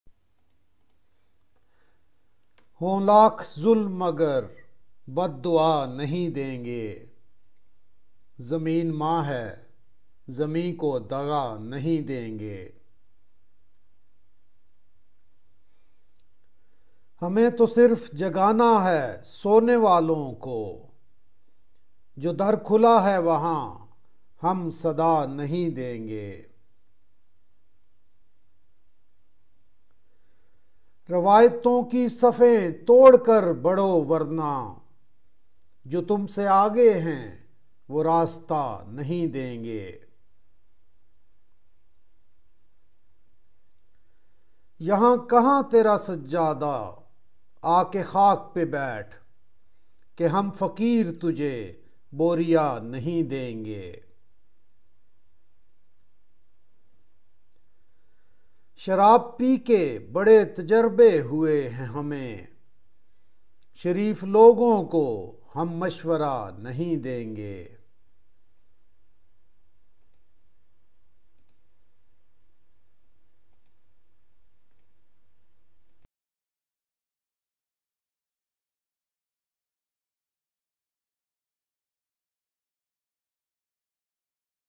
Hon lakh zulm magar, Rahat Indori ہوں لاکھ ظلم مگر بد دعا نہیں دیں گے، راحت اندوری
ہوں لاکھ ظلم مگر بد دعا نہیں دیں گے
زمین ماں ہے زمیں کو دغا نہیں دیں گے

ہمیں تو صرف جگانا ہے سونے والوں کو
جو در کھلا ہے وہاں ہم صدا نہیں دیں گے

روایتوں کی صفیں توڑ کر بڑھو ورنہ
جو تم سے آگے ہیں وہ راستہ نہیں دیں گے

یہاں کہاں ترا سجادہ آ کے خاک پہ بیٹھ
کہ ہم فقیر تجھے بوریا نہیں دیں گے

شراب پی کے بڑے تجربے ہوئے ہیں ہمیں
شریف لوگوں کو ہم مشورہ نہیں دیں گے